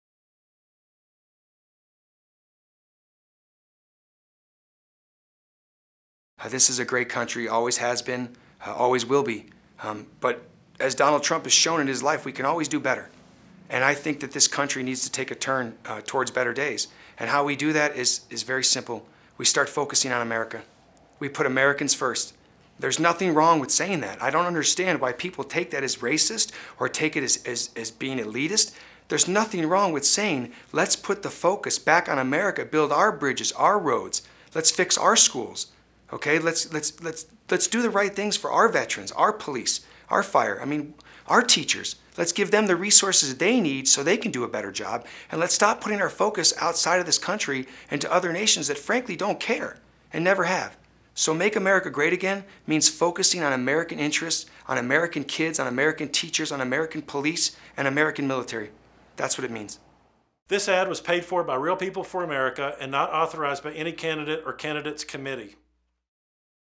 Joey Gilbert - America First
Joey Gilbert, Reno attorney, former middle-weight champion and star of The Contender, supports Donald Trump for President